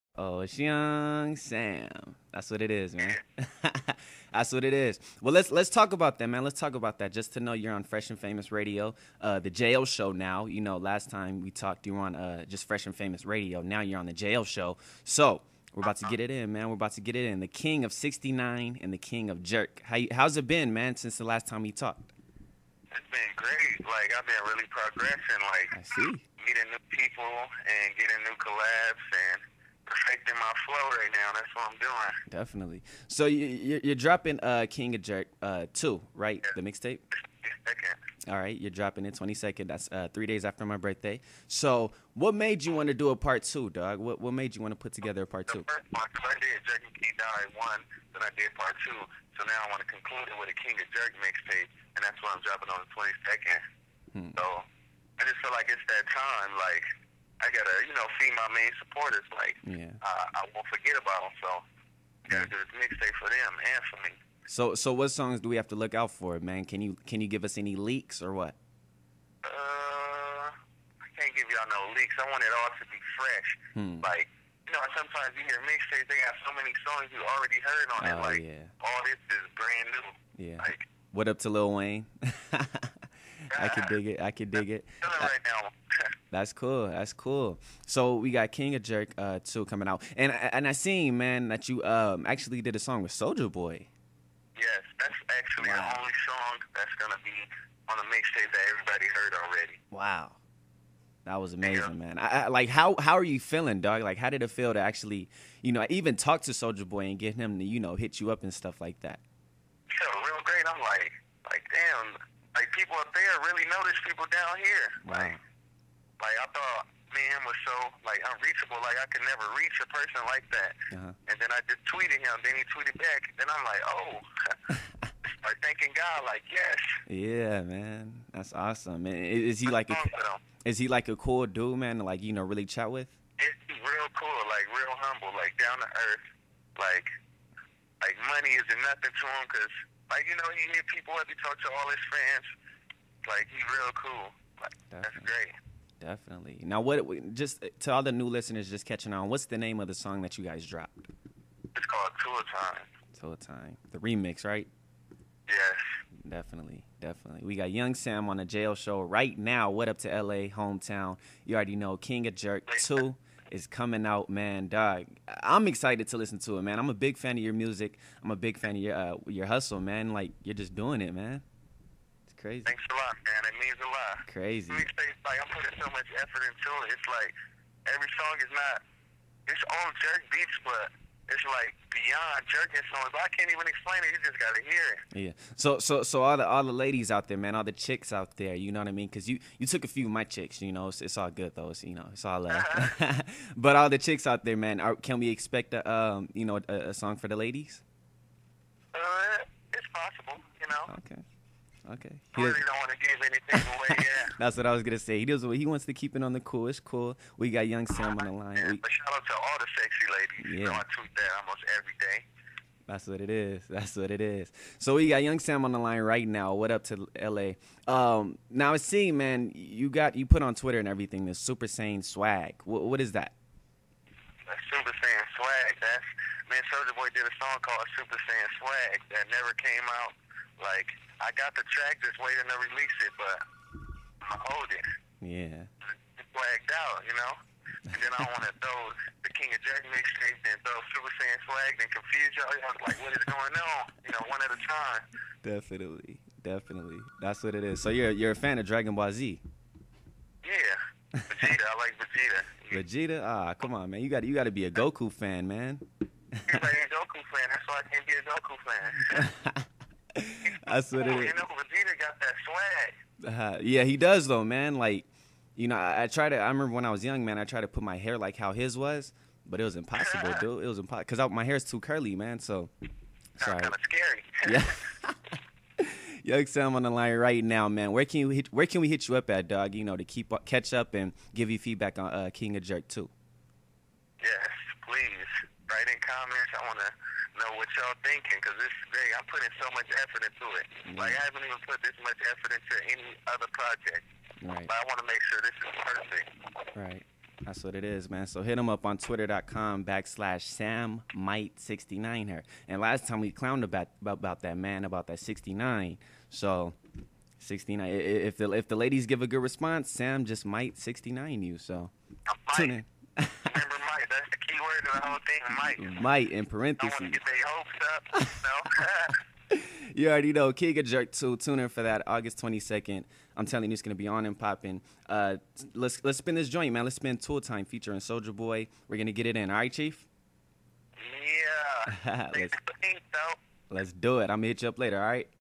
My Interview w/ Young Sam
Young Sam is a rapper, actor and entrepreneur.

Phresh N Phamous Radio (PHx2) every Friday's.

The Jay-O Podcast (TJOP) every Monday's. Stream podcast episodes now

CONQUER THE WORLD TODAY!